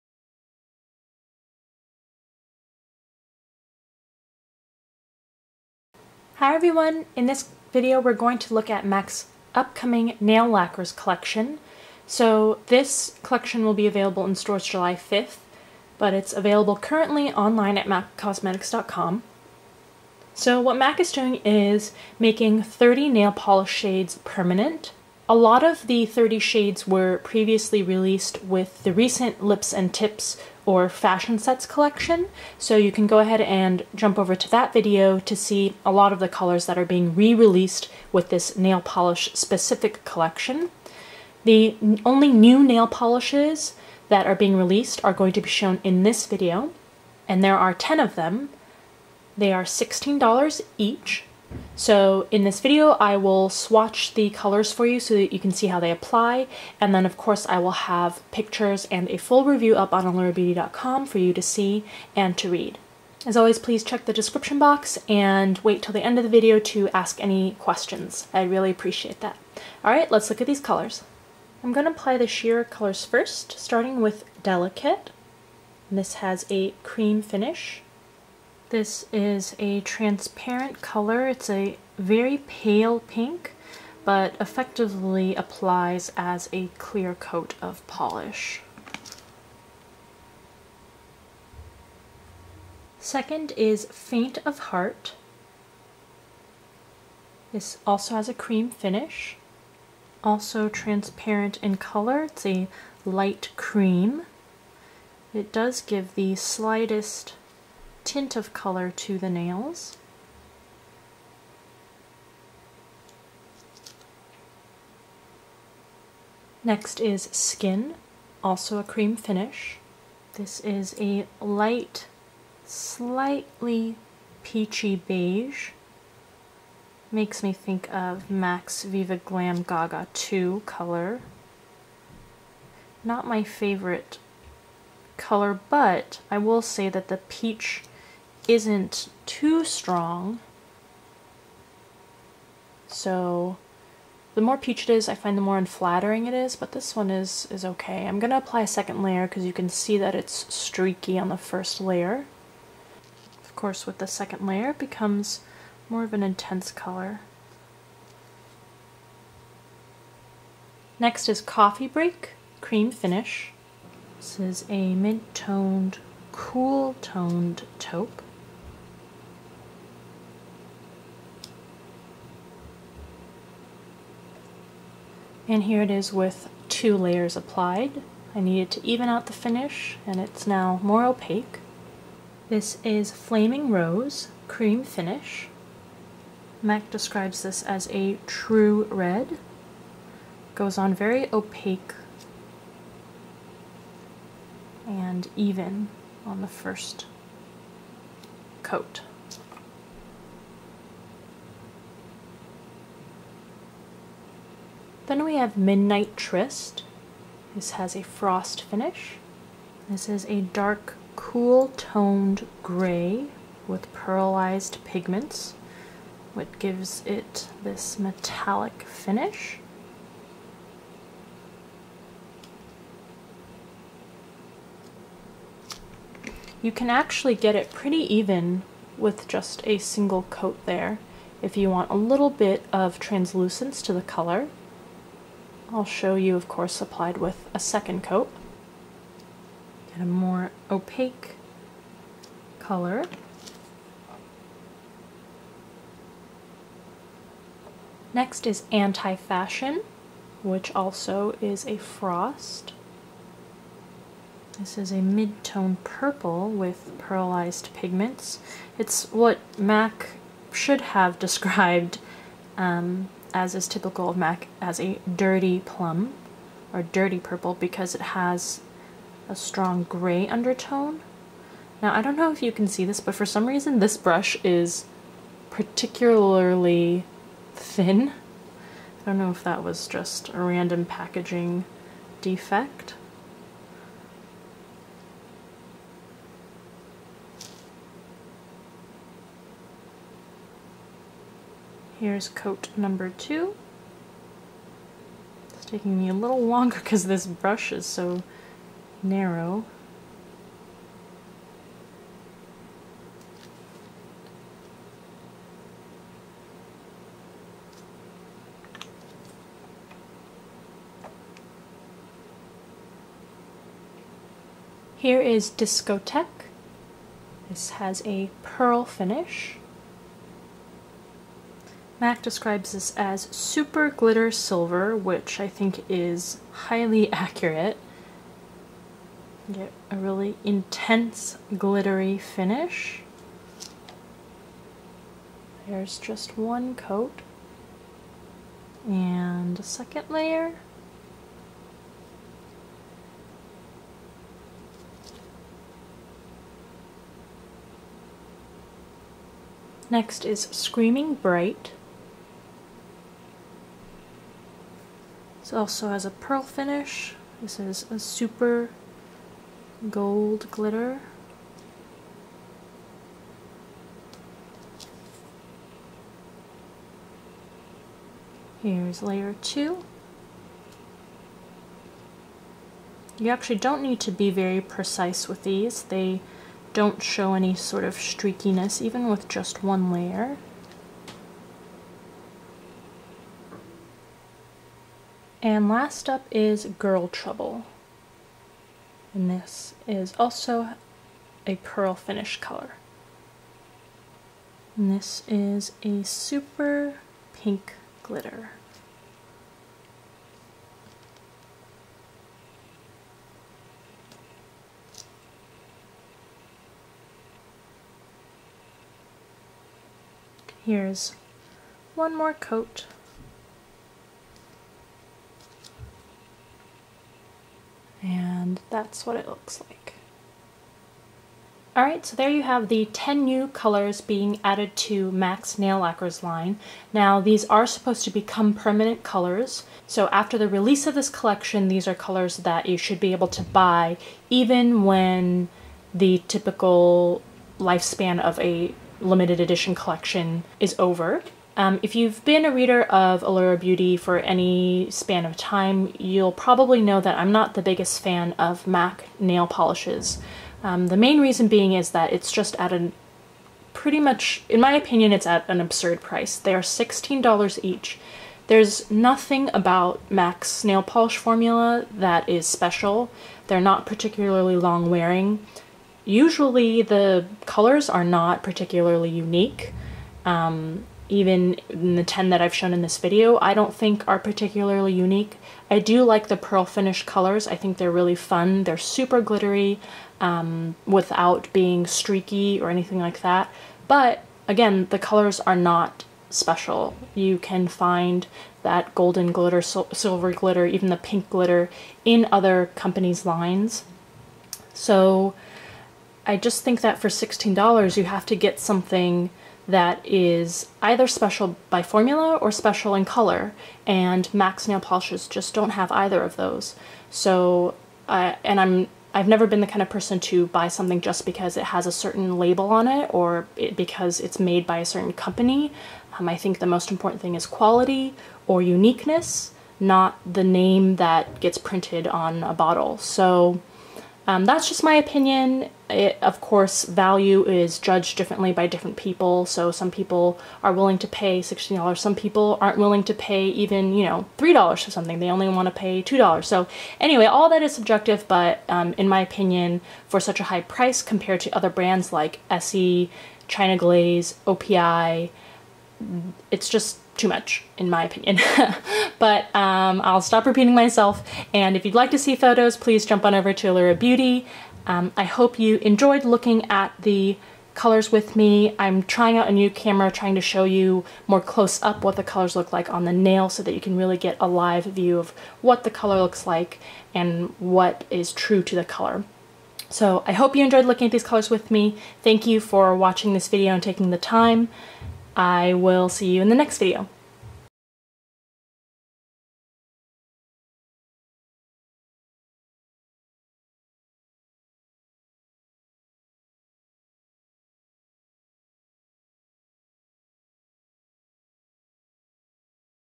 MAC Nail Lacquers Collection: Live Swatches & Review
Live swatches & review of MAC's Nail Lacquers Collection.  Releases July 5, 2012 in stores; available online now.  See below for reviews/photos!

To see the other colors re-promoted in this collection, watch the MAC Lips & Tips / Fashion Sets Collection Video